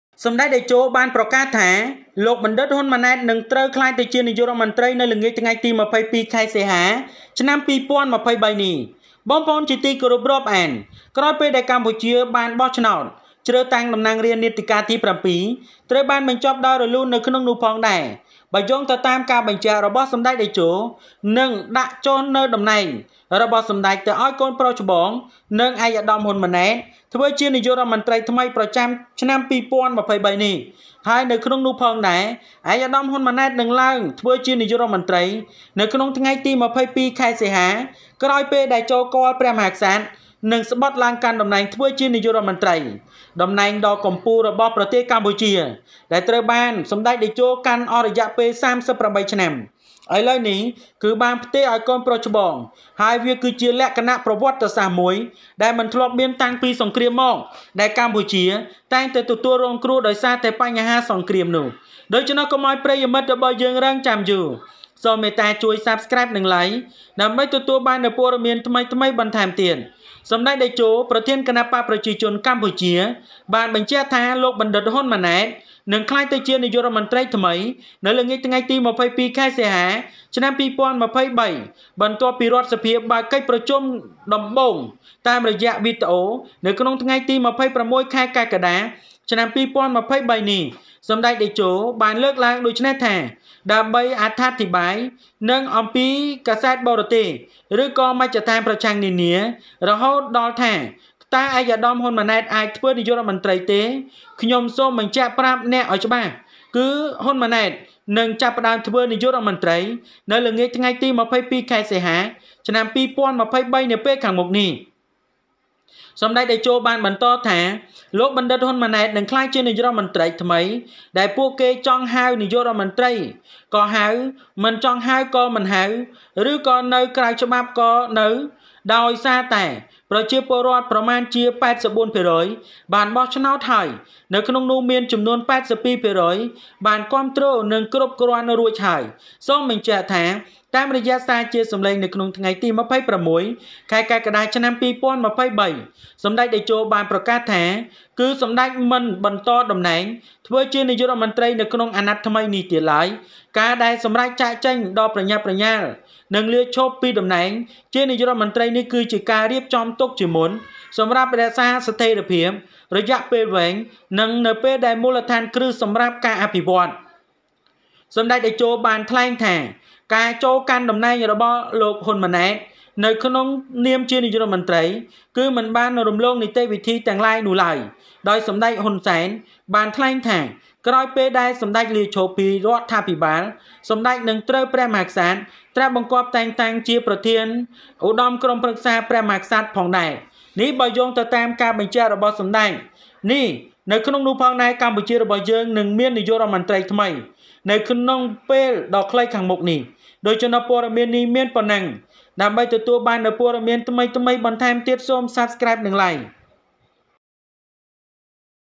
សម្តេចតេជោ ប្រកាសថា លោកបណ្ឌិត ហ៊ុន ម៉ាណែត នឹងត្រូវក្លាយជានាយករដ្ឋមន្ត្រី នៅល្ងាចថ្ងៃទី២២ ខែសីហា
សម្តេចតេជោ ប្រកាសថា លោកបណ្ឌិត ហ៊ុន ម៉ាណែត នឹងត្រូវក្លាយជានាយករដ្ឋមន្ត្រី នៅល្ងាចថ្ងៃទី២២ ខែសីហា ឆ្នាំ២០២៣ Samdech Techo announces that Dr. Hun Manet will be the Prime Minister on the evening of August 22, 2023.